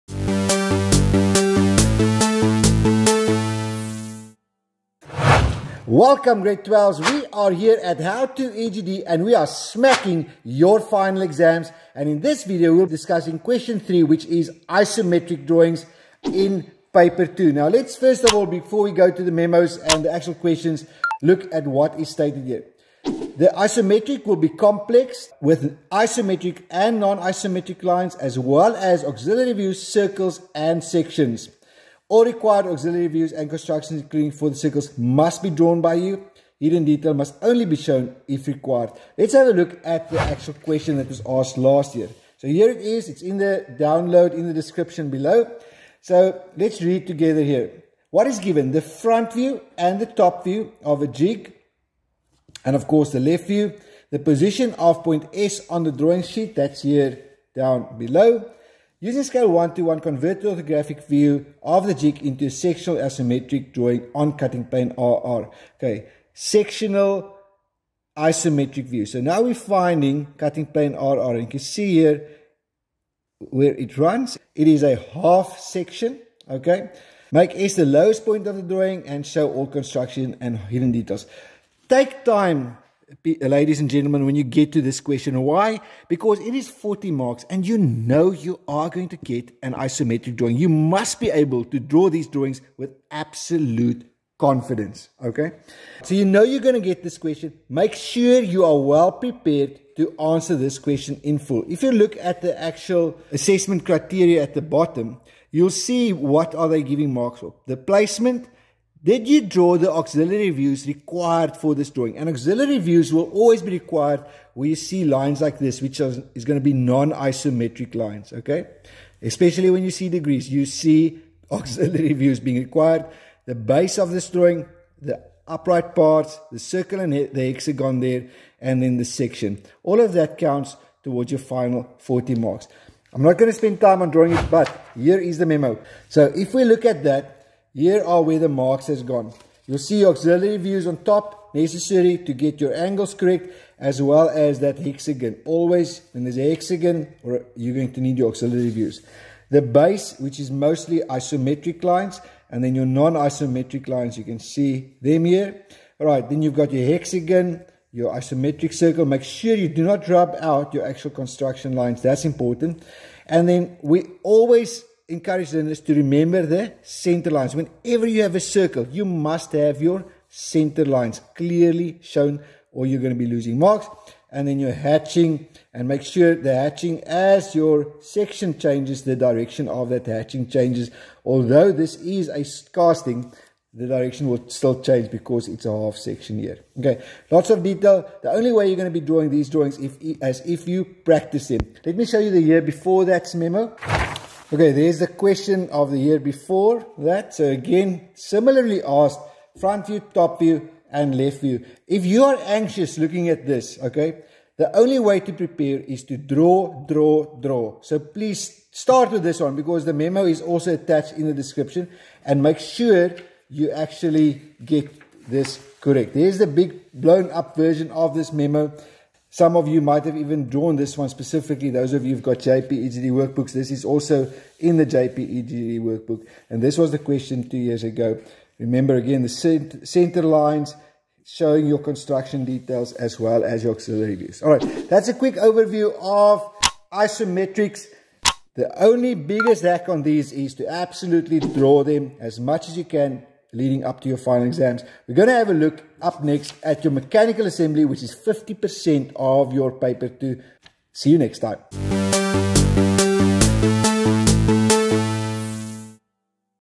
WHAT NOT TO MISS in ISOMETRIC - GR 12 - EGD
Team HowToEGD!! This is it, your last opportunity to absolutely SMACK your final exams!

I champion you and I know that with determination and hard work you can absolutely SMACK your upcoming Prelims and Finals.

My FINAL advice to you: Draw - Draw - Draw as many drawings you can under similar time constraints to what you will have in the exams.

SUGGESTED TIME BREAKDOWN FOR PRACTISE DRAWINGS
15min for Analytic Questions
30-35min for all other drawings such as Isometric, Two Point, Etc.
90min for all Civil and Mechanical Assembly Drawings

You will not have more time than what I am referring to above, therefore, please do not in your preparation use more time than stated above. You have to practice until you can finish these drawings within the given time frames,

PREVIOUS EGD EXAM PAPERS - FREE DOWNLOADS WITH MEMO'S

Now it really is your turn.